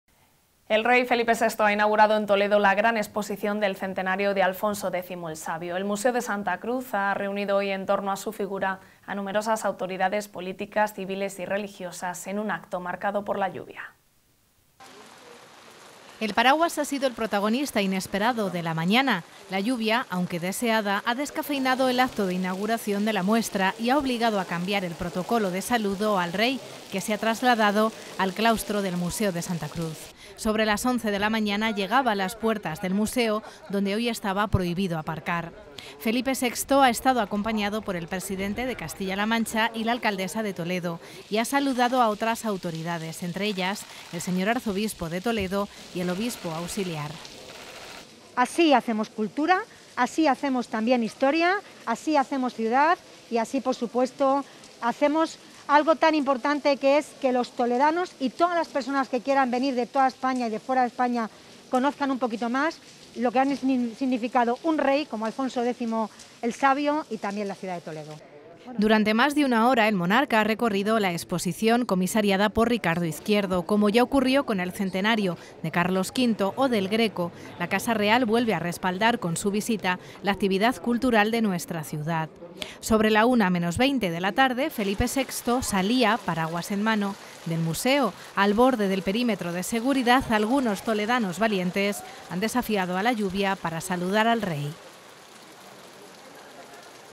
Felipe VI inaugura la exposición del centenario de Alfonso X
El rey Felipe VI ha inaugurado en Toledo la gran exposición del centenario de Alfonso X El sabio. El Museo de Santa Cruz ha reunido hoy en torno a su figura a numerosas autoridades políticas, civiles y religiosas en un acto marcado por la lluvia.